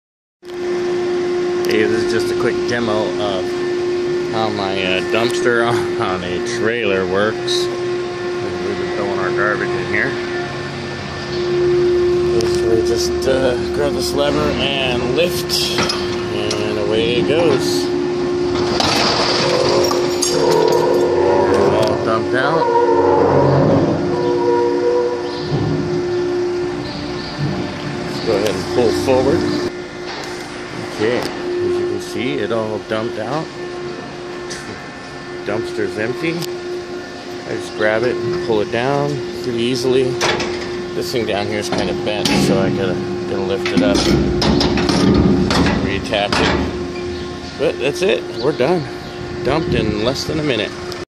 Self dumping hopper on trailer frame dump truck trash fast
This is a demonstration of how speedy a dump run can be